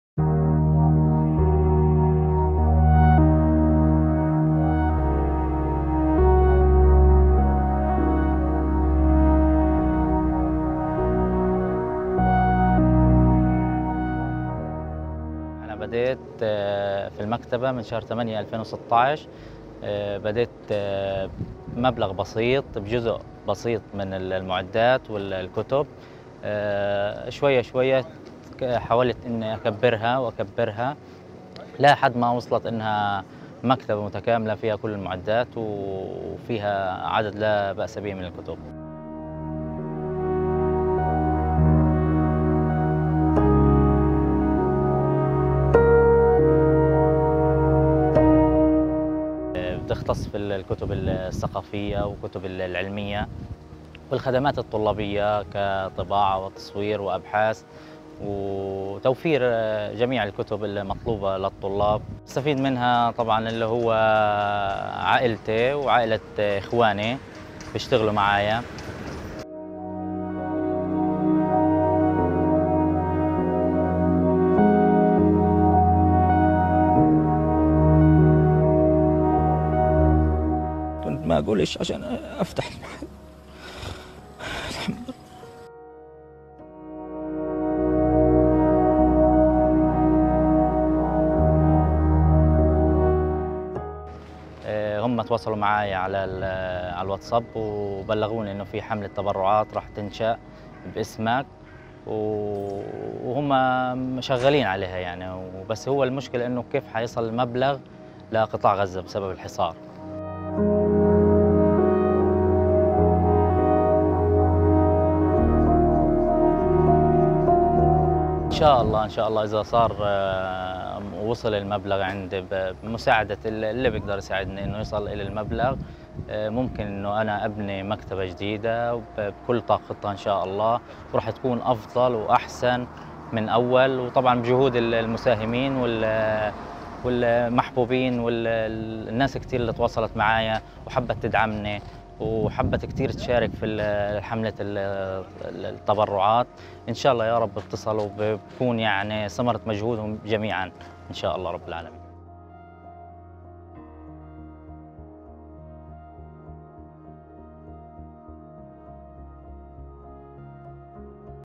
Owner of bombed bookstore in Gaza feels hope after $100,000 fundraiser
Shaban Aslim’s emotional interview went viral after Israeli air strikes destroyed his family’s bookstore in Gaza.

Moved by his story, an online campaign has raised $130,000 to help him rebuild his business, giving hope that his dream can still survive.